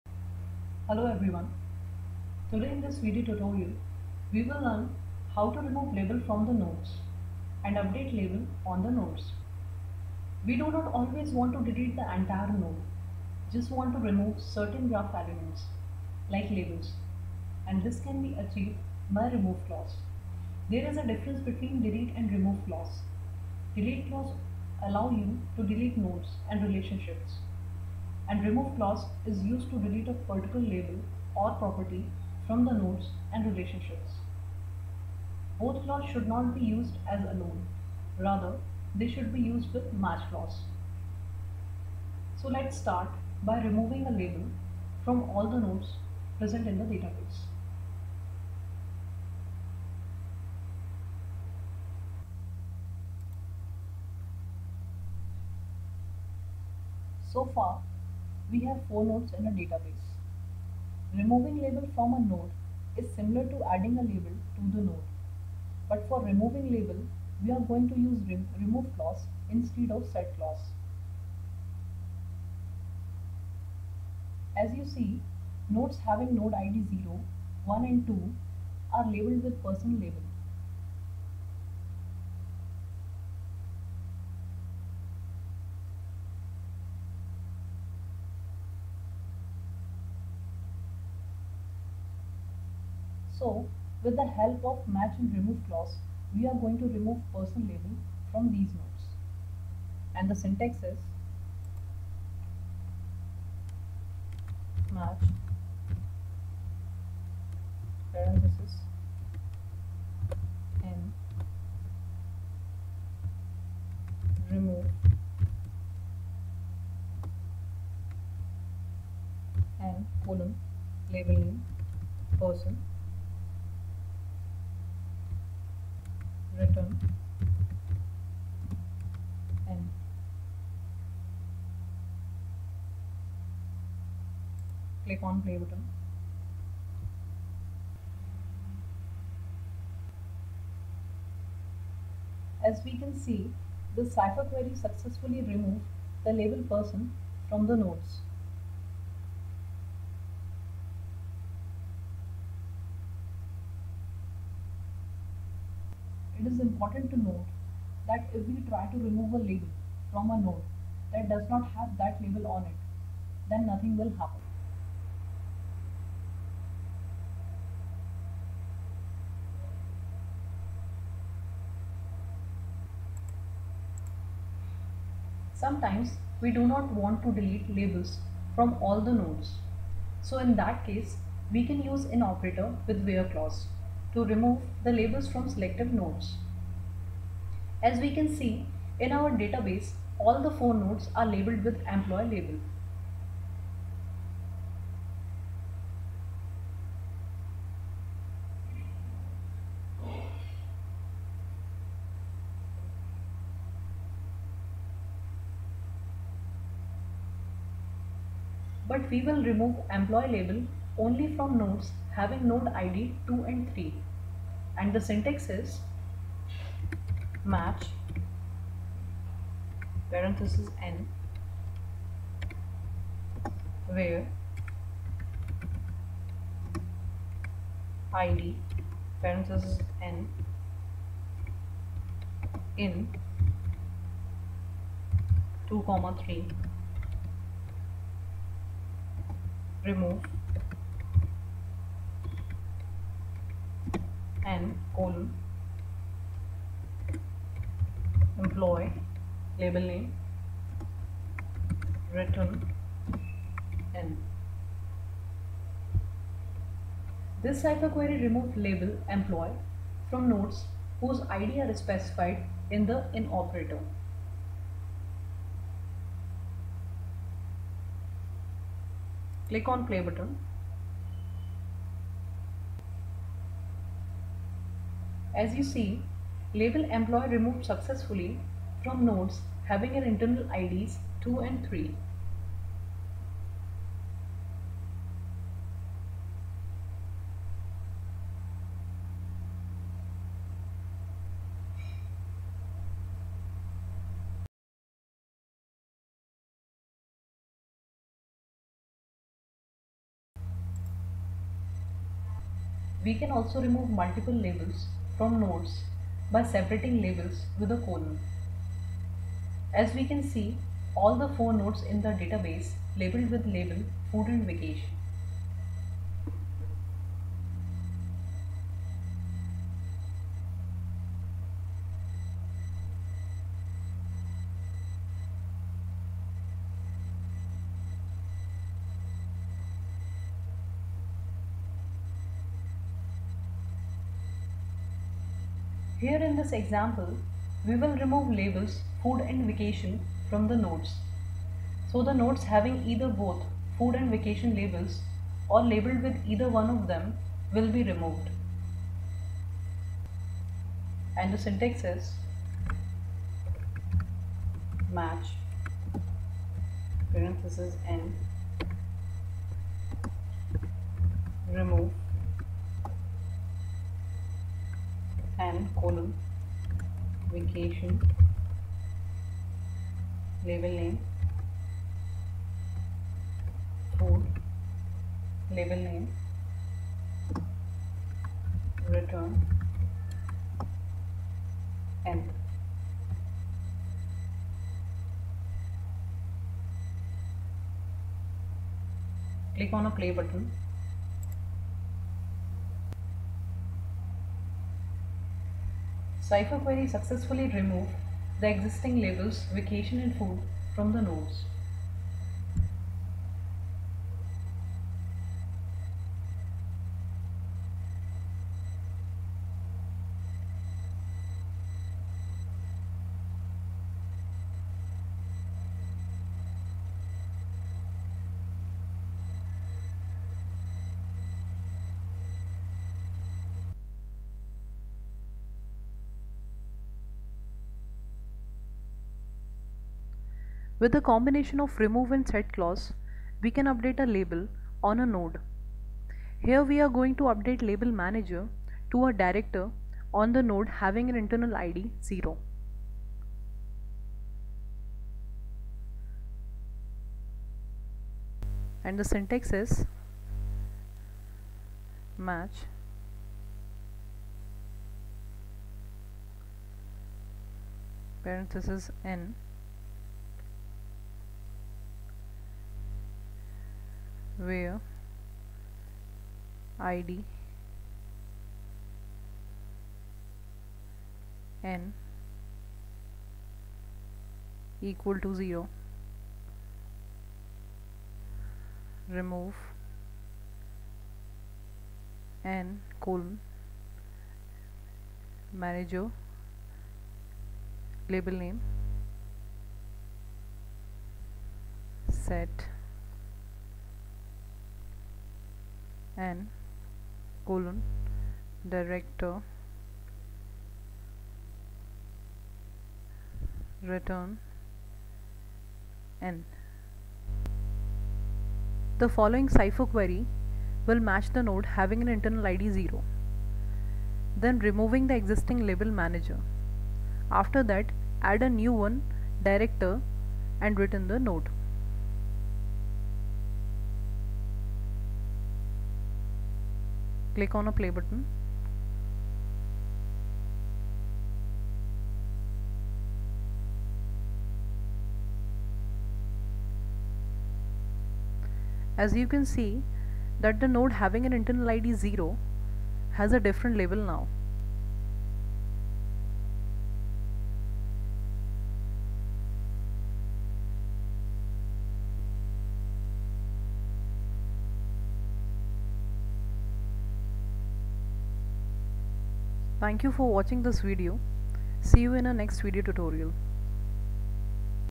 Neo4j Tutorial 7 : Remove and Update Labels on Nodes
In this video tutorial we will learn how to remove label from the nodes,update label on the nodes.